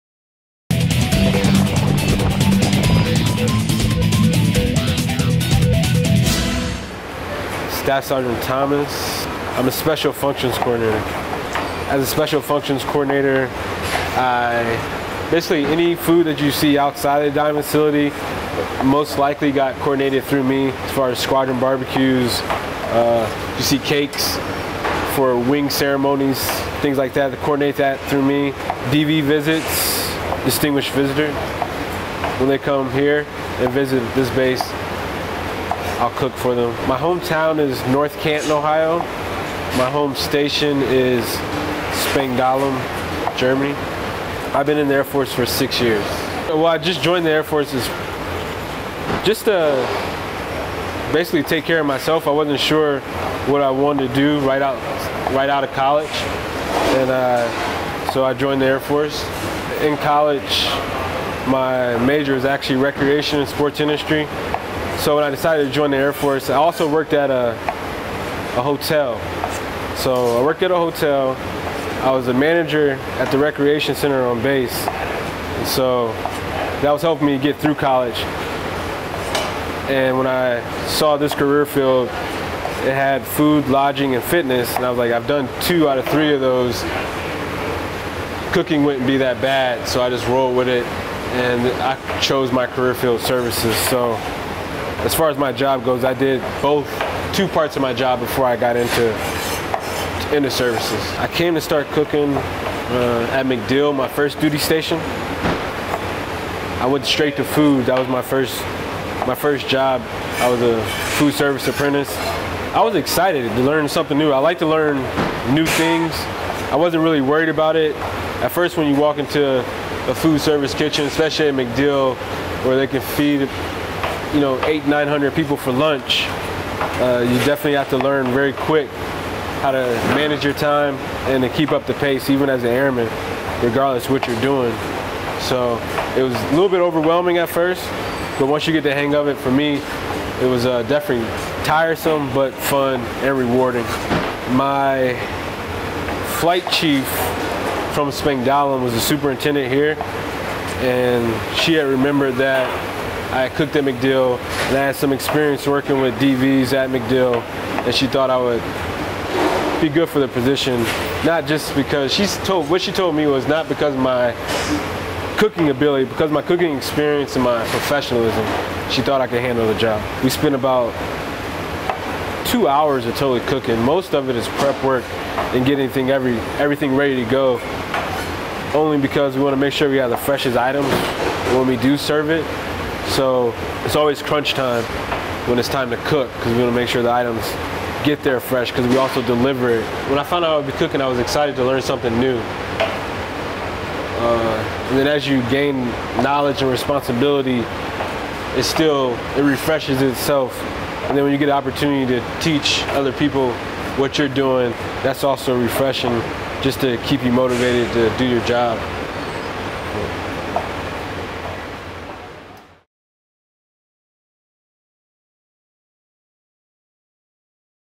MilitaryChefs.com - Airmen in the Fight: Staff Sgt. Deangelo Thomas
- Staff Sgt. Deangelo Thomas was recently selected as one of Air Force Central Command's "Airmen in the Fight" for 2013. This video showcases SSgt Thomas' job as a special functions coordinator for the 376th Expeditionary Force Support Squadron. Also available in high definition.